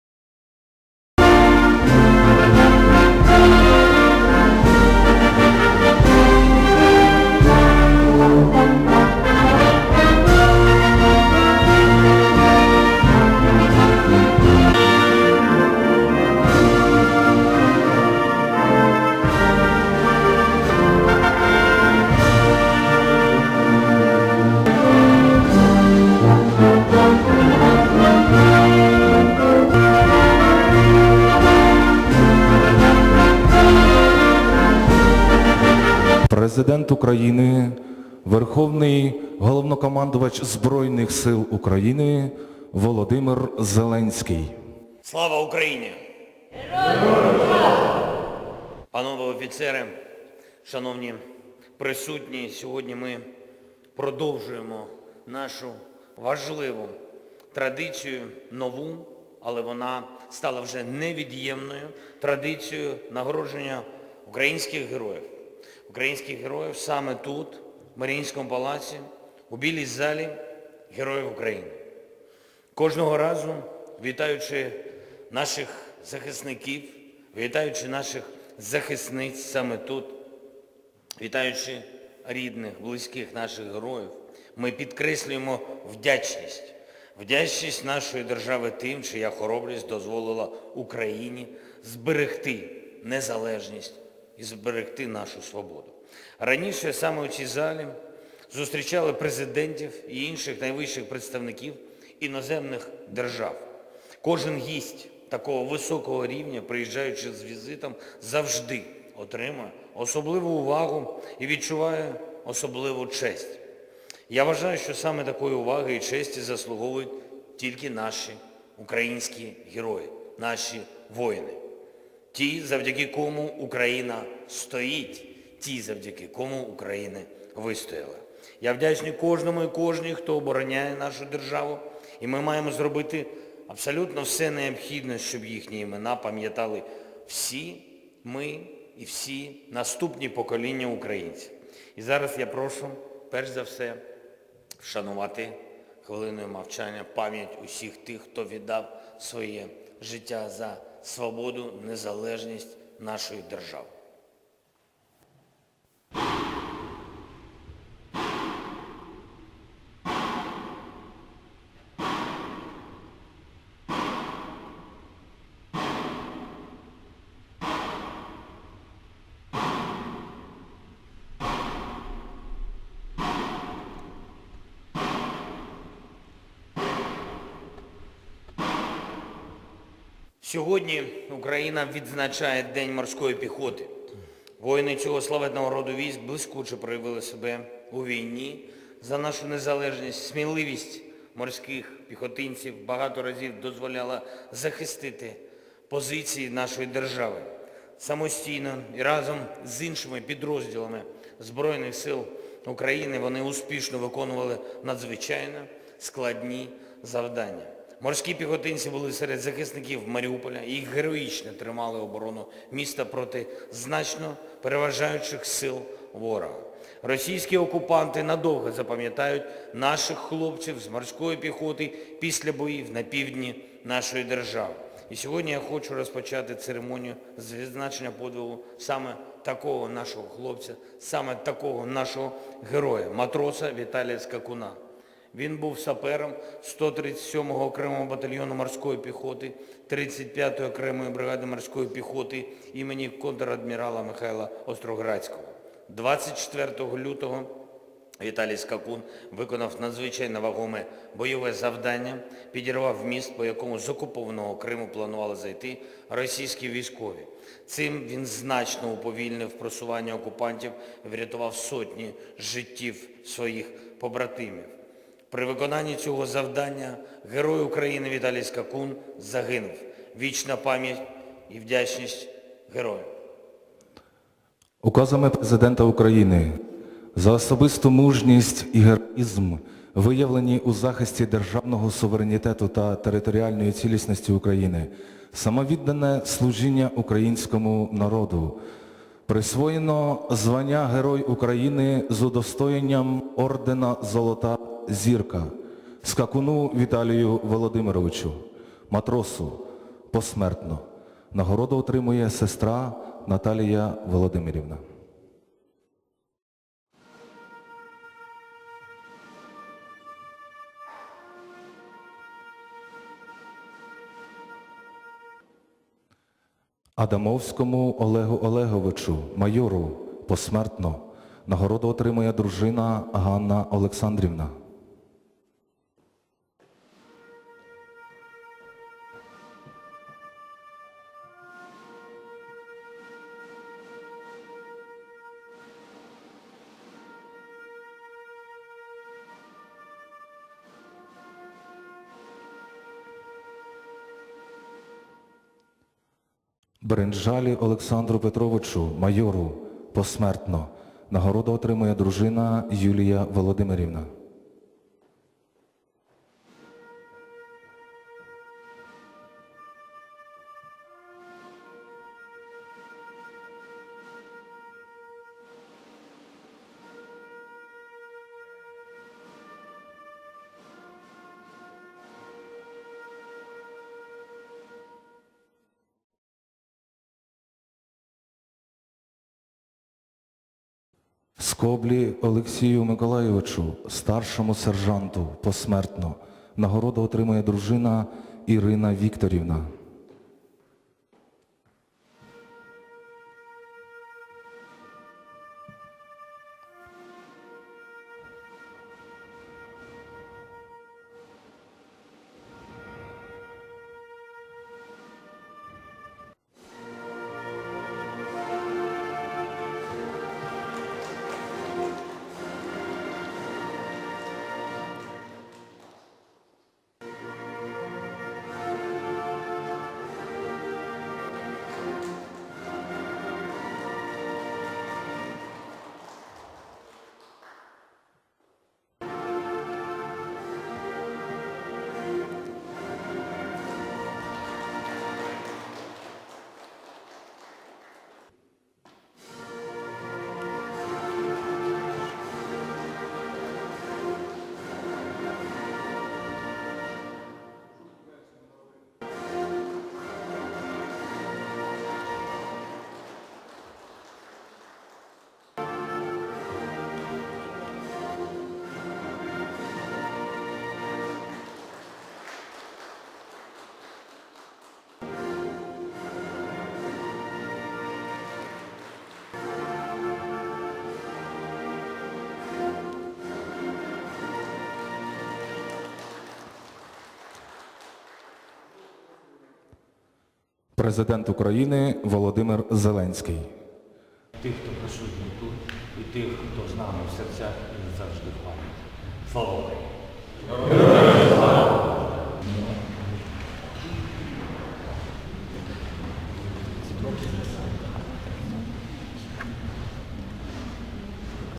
Zelensky presented orders to servicemen and relatives of the fallen defenders
Зеленський вручив ордени військовослужбовцям і рідним загиблих захисників,